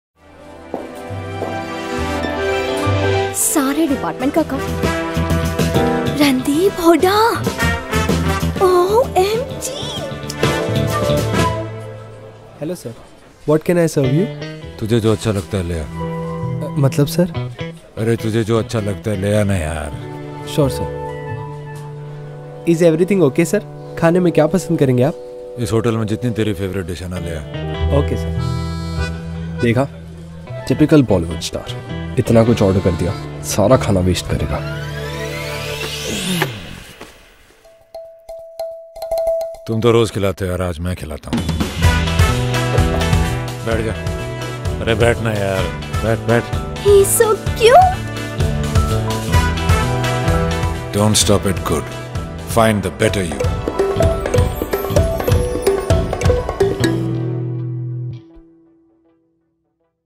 BetterU Shaving Gel (TVC)
T V Commercial of BetterU Shaving Gel by Bollywood Actor Randeep Huda